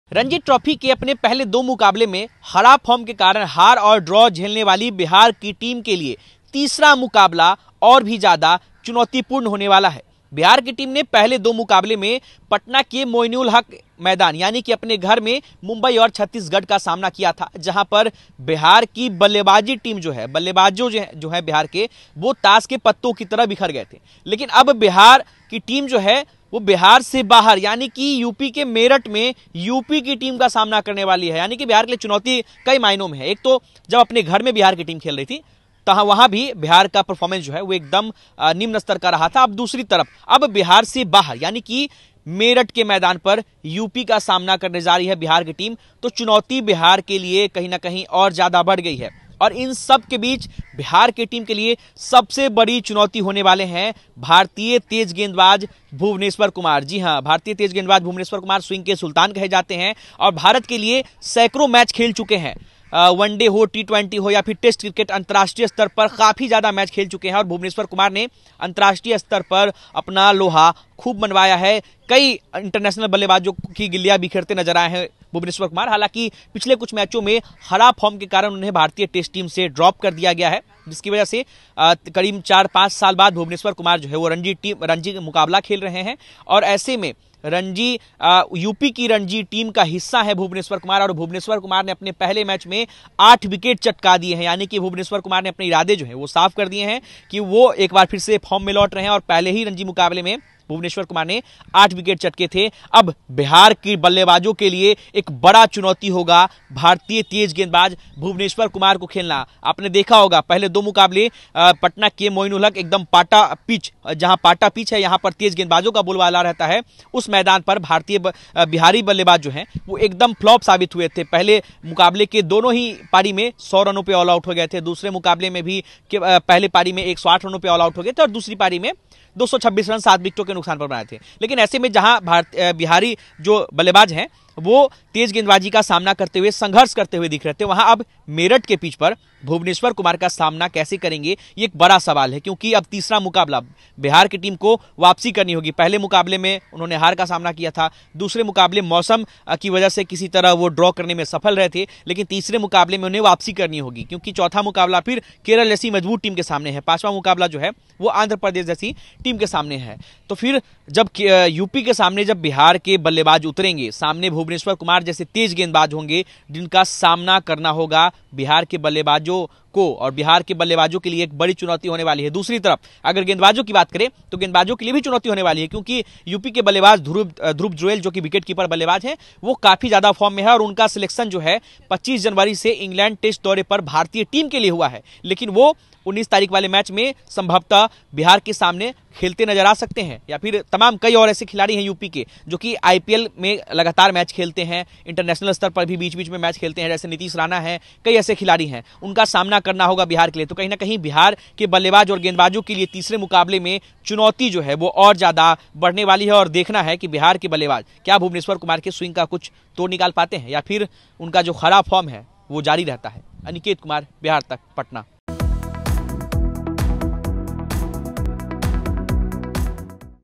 Ranji Trophy UP Vs Bihar : Bhuvneshwar Kumar के स्विंग के सामने होगी Bihar की अग्नि परीक्षा
रणजी ट्रॉफी के तीसरे मुकाबले मे भारतीय तेज गेंदबाज भुवनेश्वर कुमार के स्विंग का सामना कैसे करेंगे बिहार के बल्लेबाज,19 जनवरी से मेरट मे यूपी से मुकाबला।